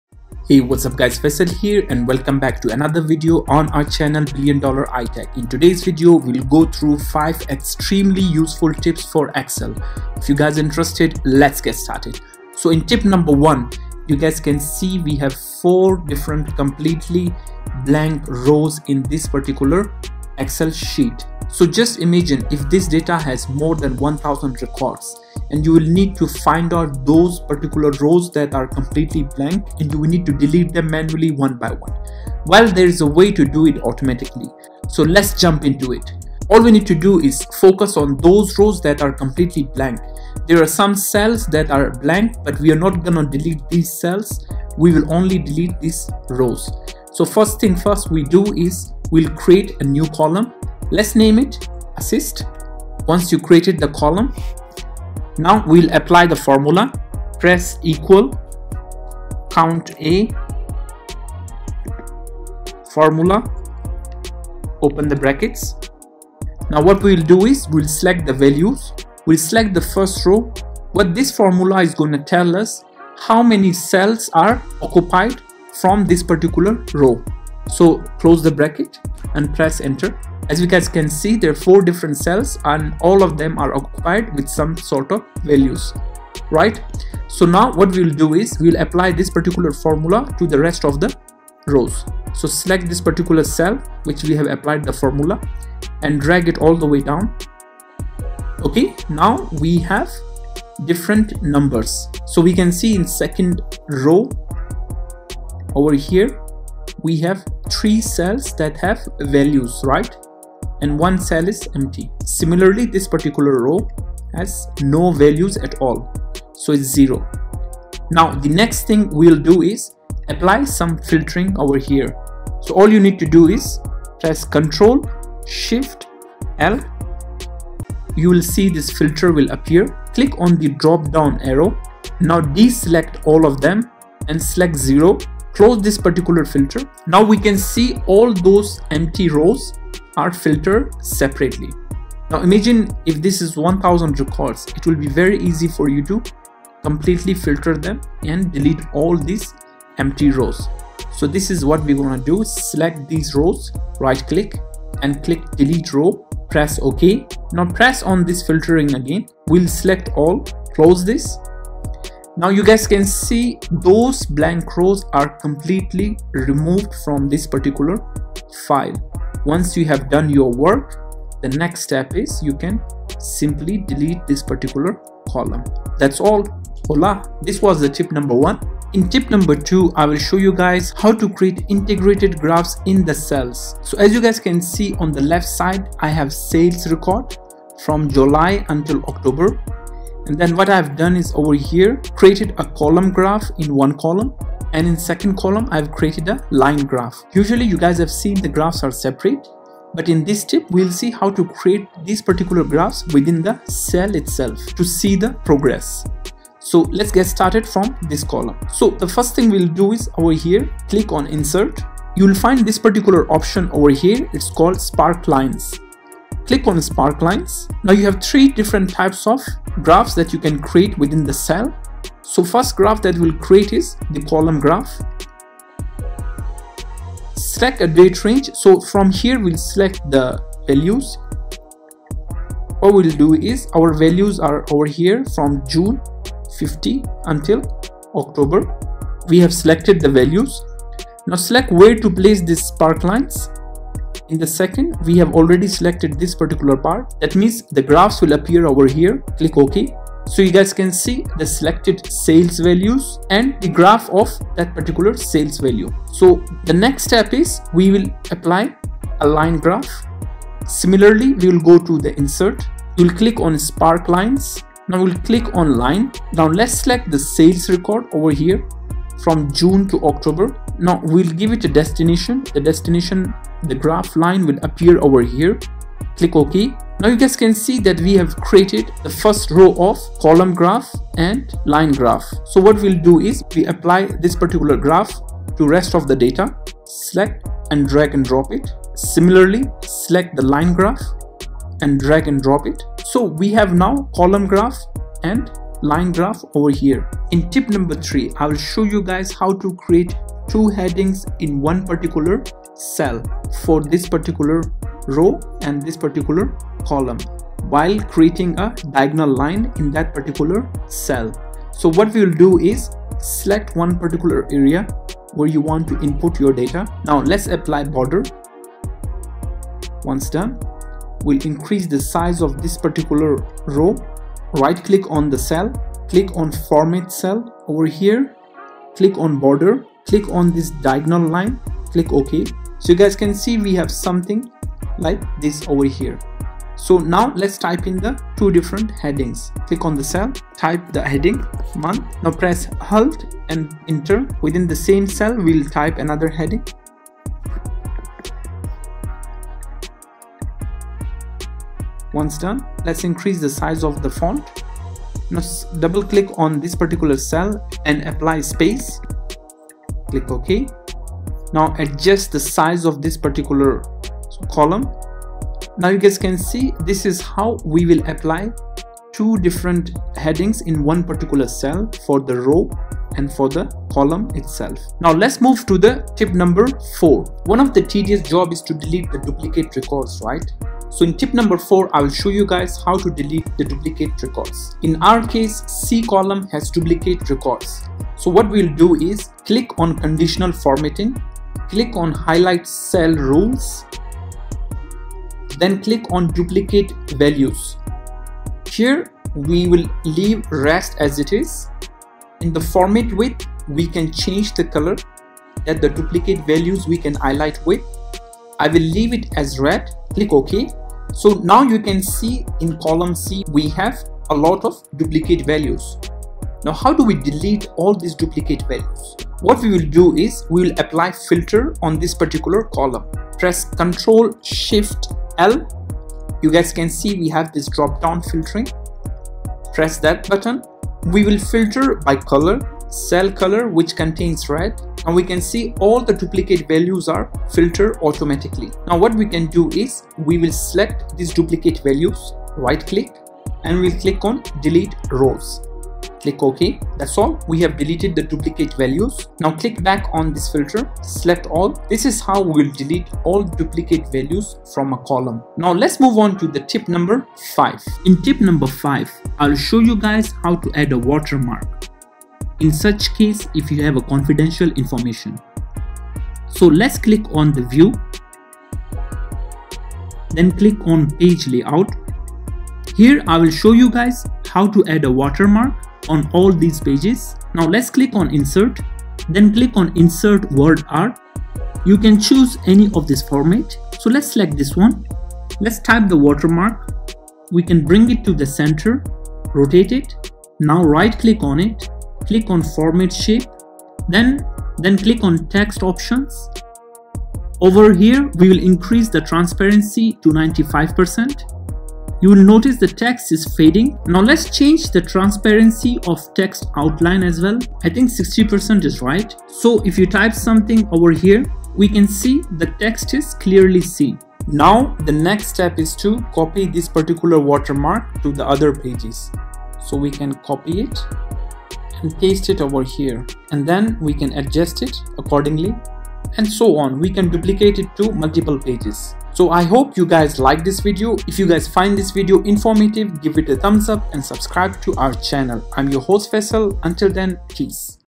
5 Excel Most Extremely Useful Tips Every Excel User Must Know
Microsoft Excel most useful tips that everyone should know. I will share five best and most useful Important Excel Tricks and Excel Tips that every Excel geek should know.